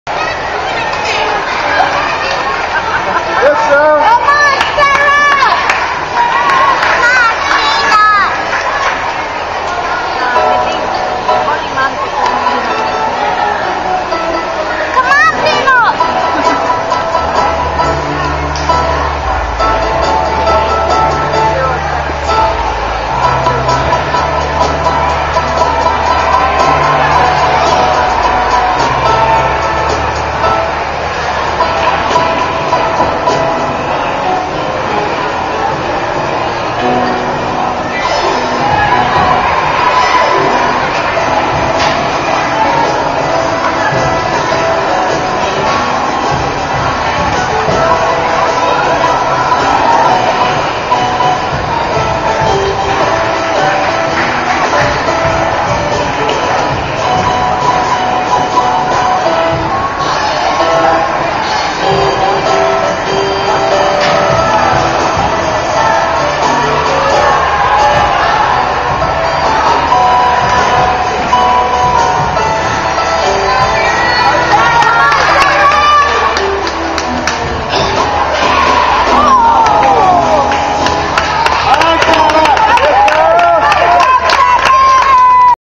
Sarah C Aviator Gymnastics:2012 Manhattan Classic Level 5 Floor Exercise
Sometimes she doesn't know her own strength lol!  She had so much power to her punch she probably could have thrown another handspring or tuck.  I love how she was able to laugh at herself after and the reaction of the audience is just hysterical.